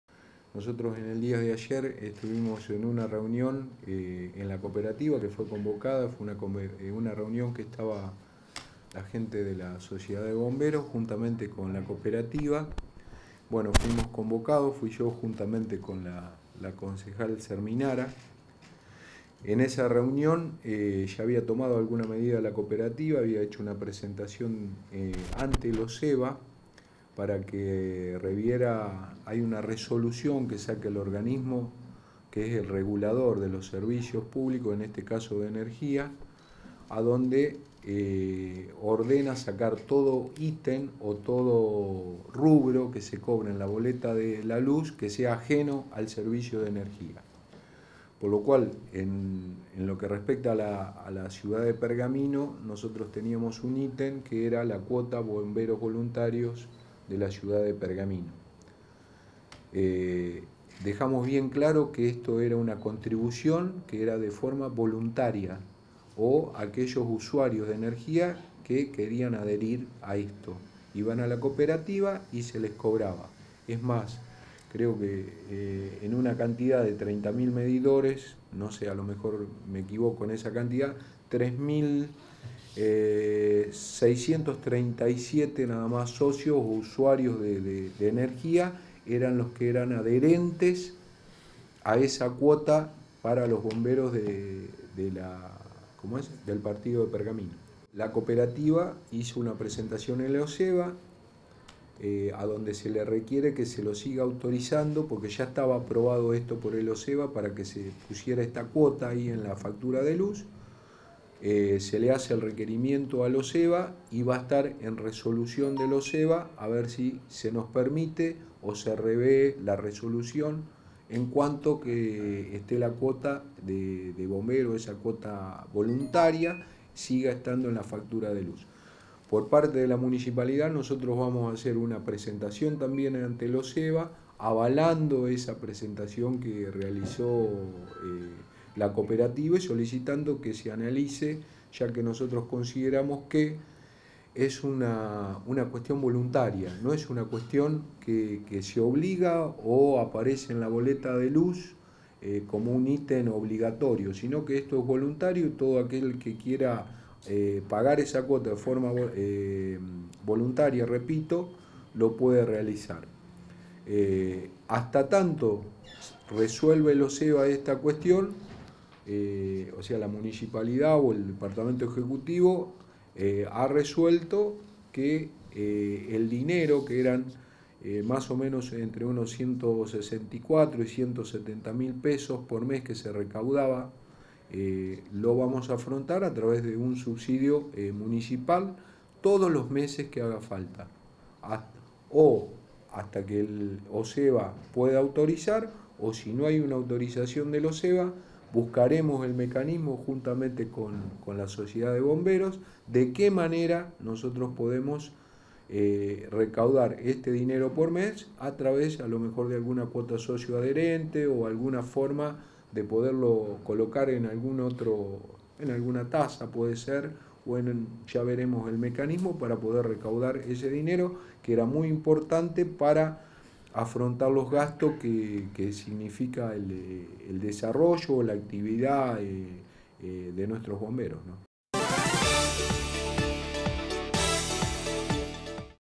El Municipio subsidiará a los bomberos.
Frecuencia 7 Cablevisión Pergamino y a la región por Canal 6 de Acevedo.